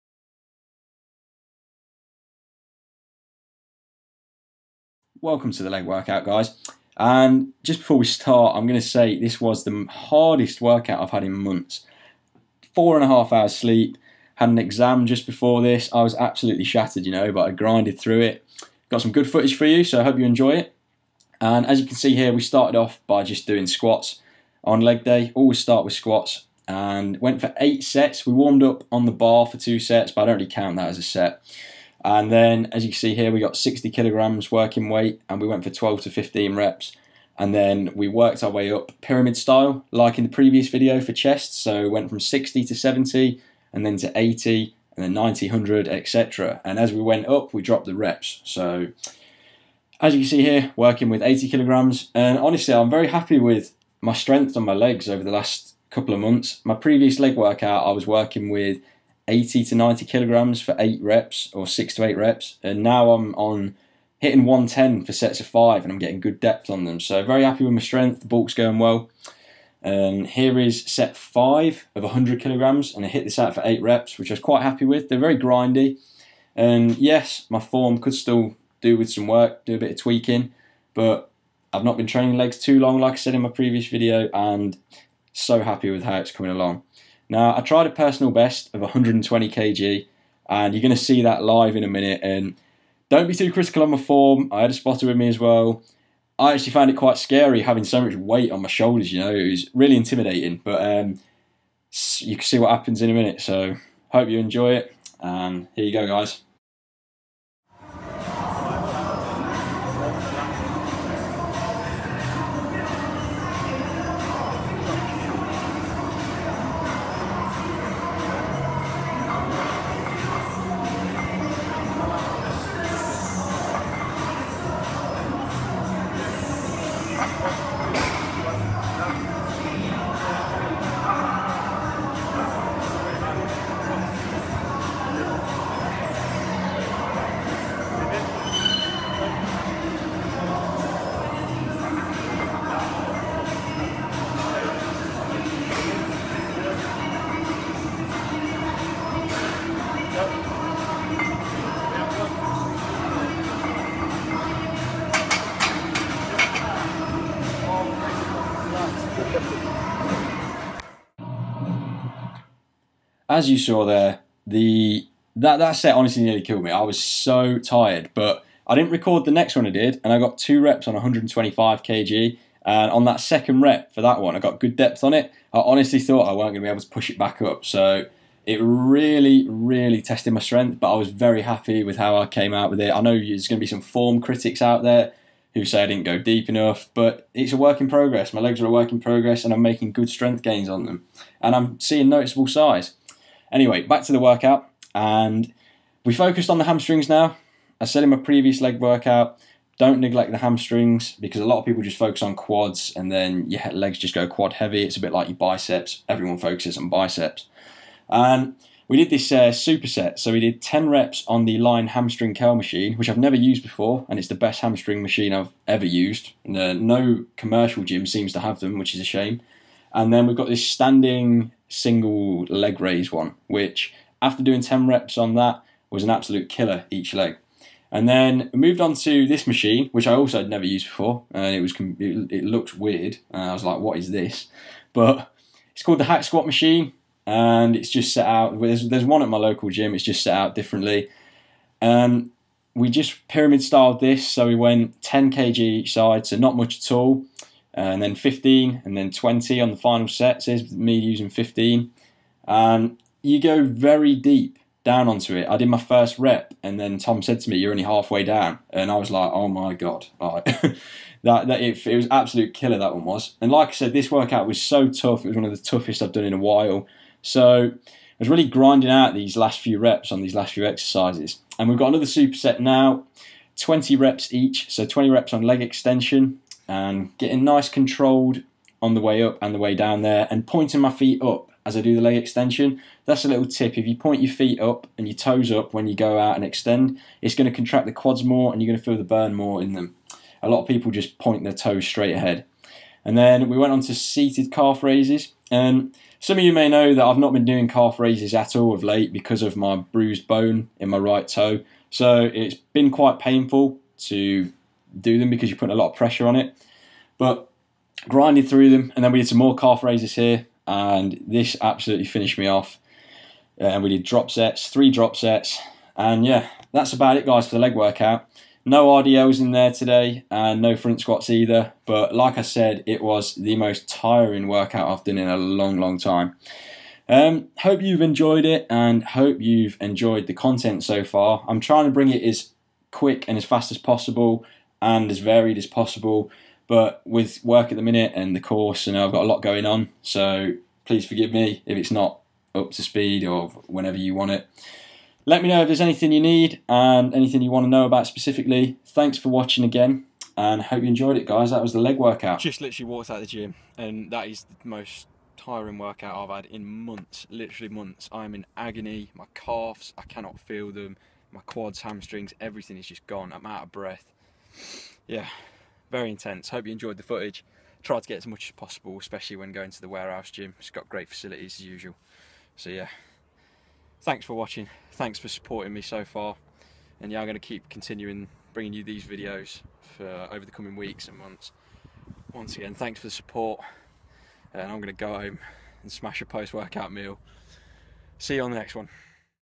LEG DAY @ WAREHOUSE
Hit an intense leg session last week, managed to hit a new PB on Squats and that was running off 4 hours sleep. Very happy with my leg progression but still working on my form! Hope you enjoy this short video and let me know what you think!